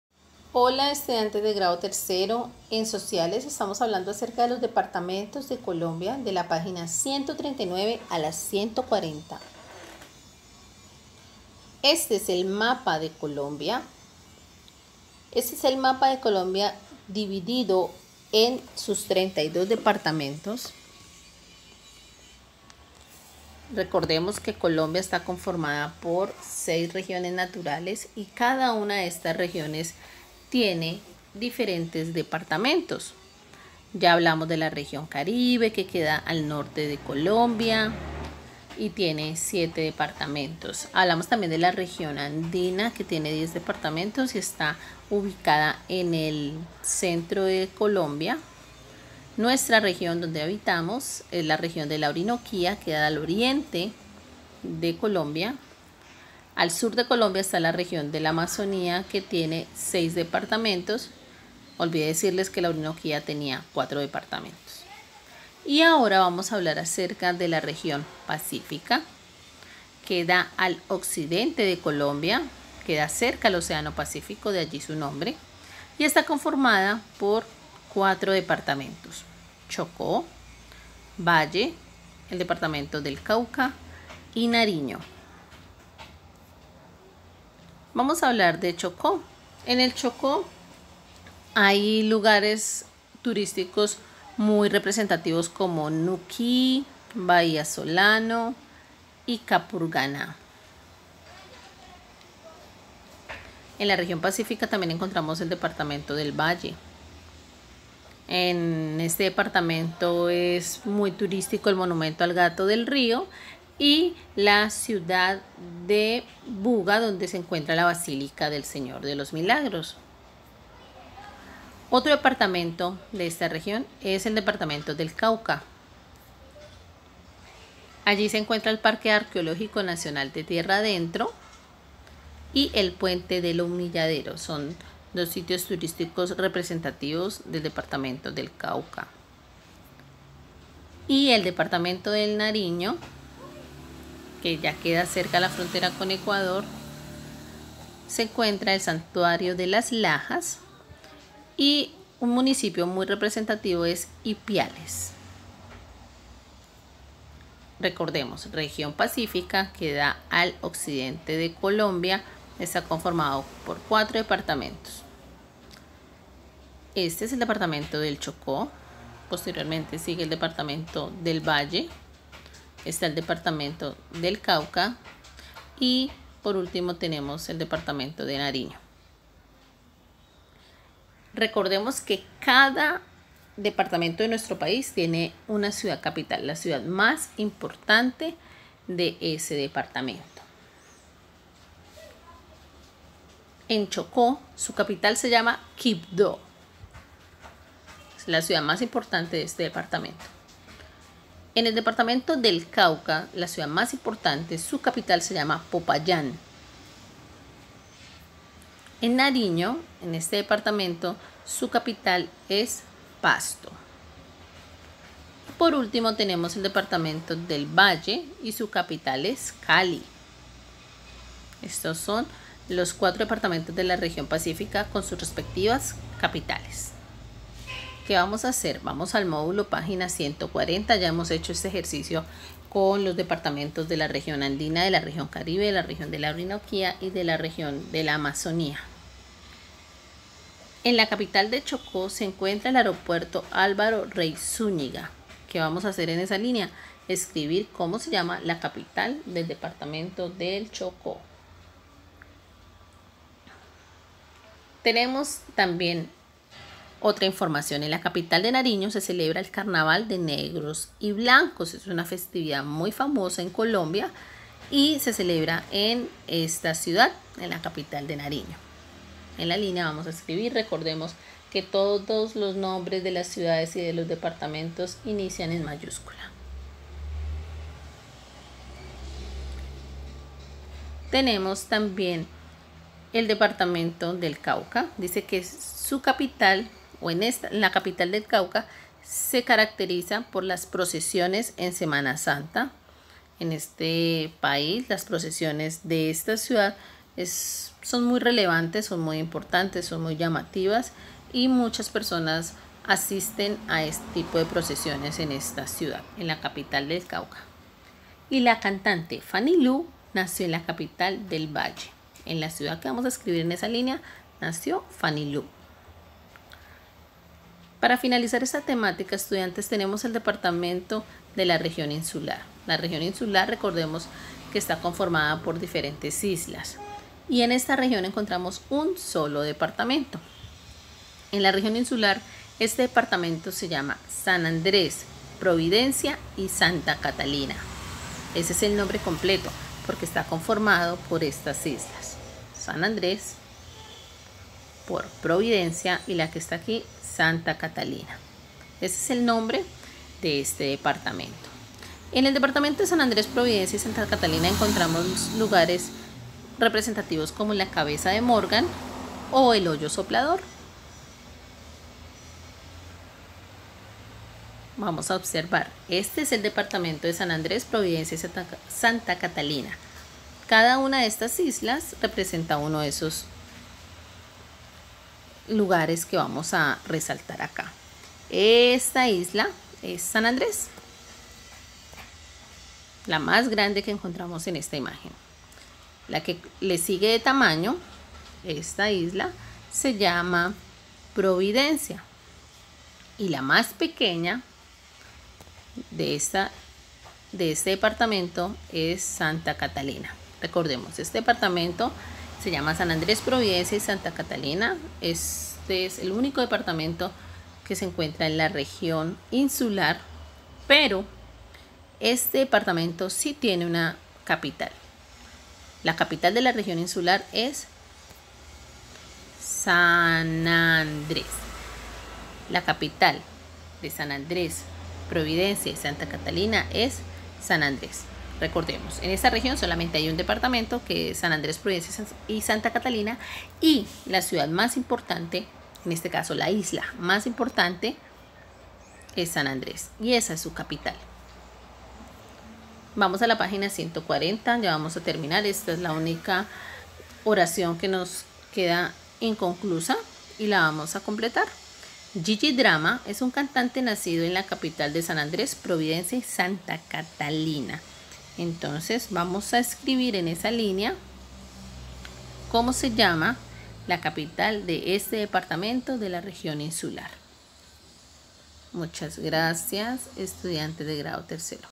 Región Pacífica e Insular departamentos y capitales
Título: departamentos y capitales de la región Pacífica e Insular
Materia: Sociales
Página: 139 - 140
Tema: departamentos y capitales de Colombia.
Grado: tercero